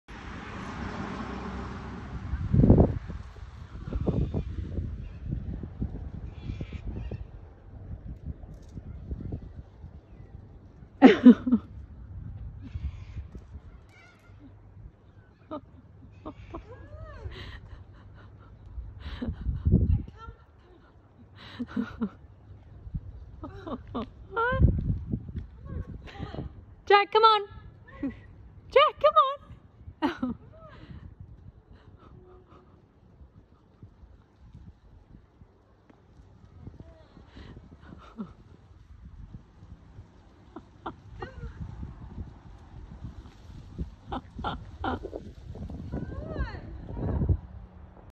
Golden Retriever Has Decided the Walk is Over || ViralHog
Occurred on March 1, 2021 / Perth, Australia

My partner Nicole and I take our golden retriever dog “Jack” for walks daily - and this video was taken 5 mins in when he decided he didn’t want to walk anymore. He’s a lazy boy and also stubborn when he doesn’t want to go a certain way - just stops and won’t move.